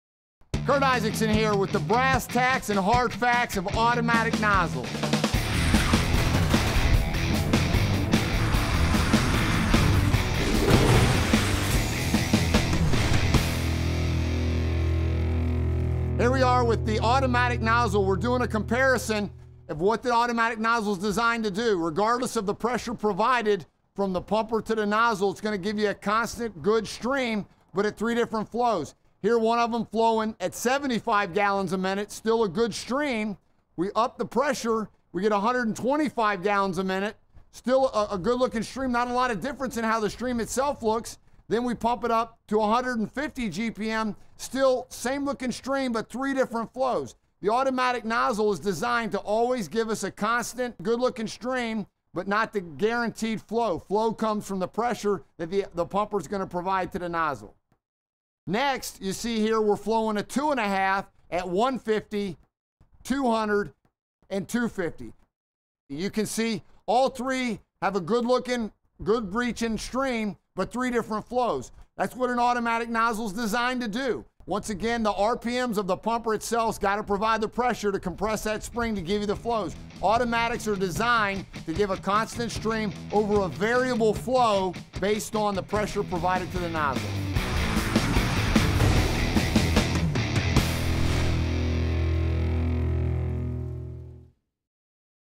Automatic Nozzles Explained with Curt Isakson - Brass Tacks & Hard Facts (Episode 10)
The automatic nozzle is designed to always give you a good looking stream over a variable flow based on the pressure provided to the nozzle. In this video, Curt Isakson gets down to the Brass Tacks and Hard Facts of automatics, giving you a side-by-side visual of their streams at a variety of flows.